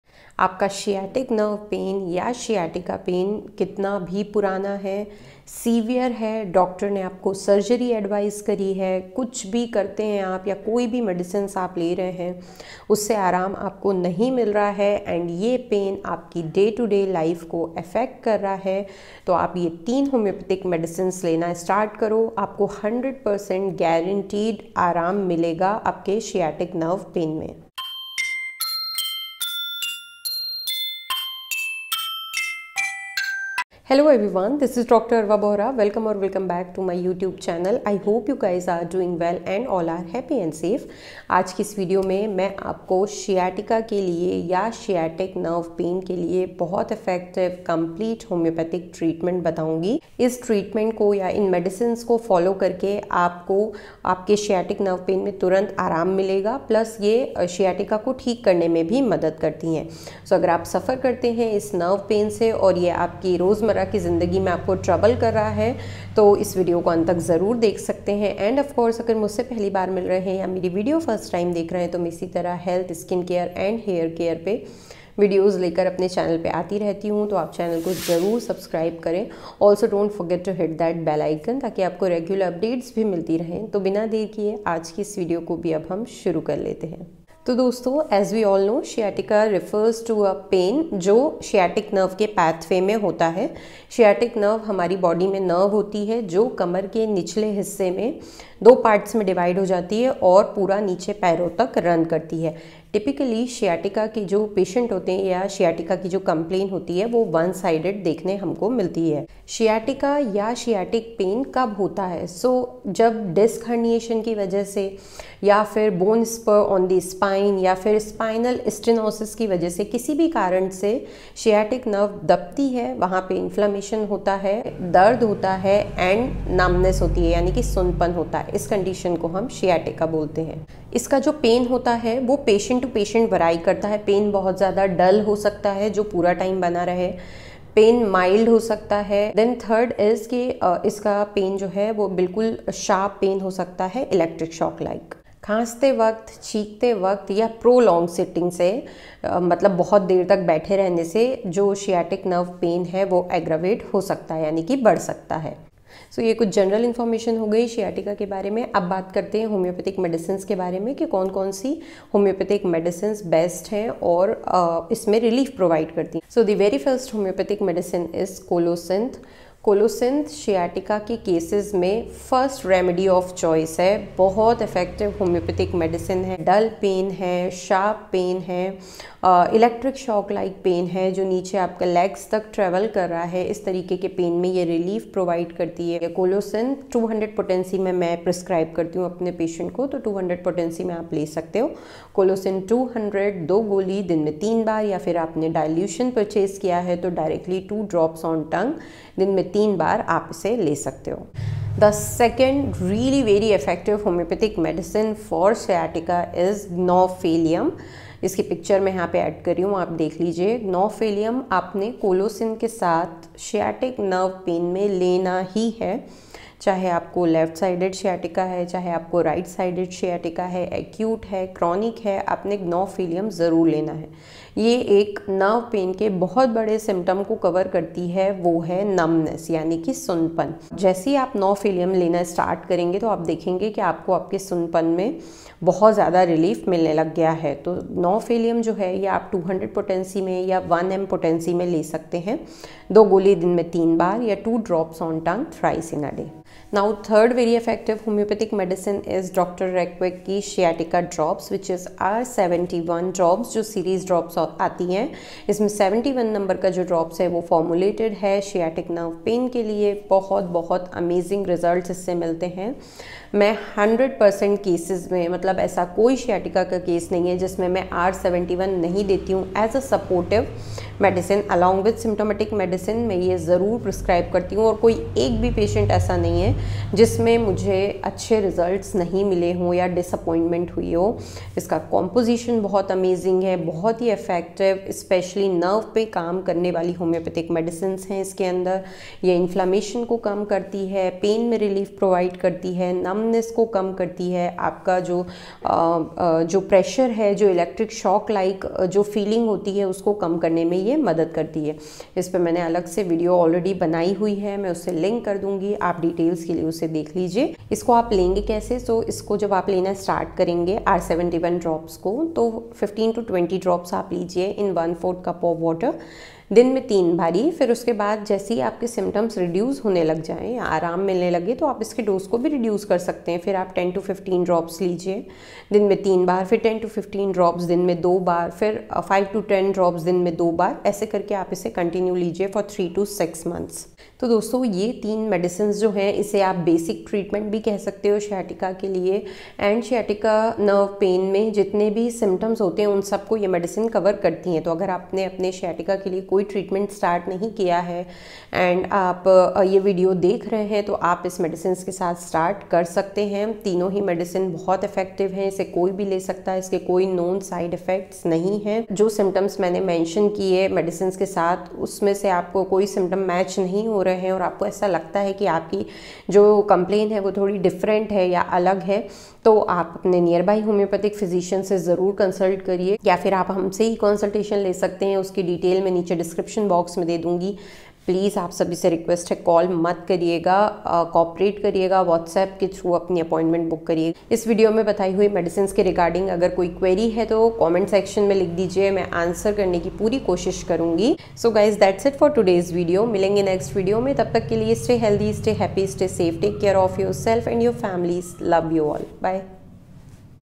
Sciatica में तुरंत और हमेशा के लिए आराम ॥Homeopathic Medicine to cure Sciatica Pain: WITHOUT SURGERY
Hello everyone, this is Dr. Arwa Bohra wellcoming you all in my youtube channel.

About this video: in this video i have shared the best effective homeopathic medicine for sciatica pain relief. Homeopathic medicines discussed are 100% effective in treatment of sciatica without surgery. Watch video for complete details.

Dr. Arwa Bohra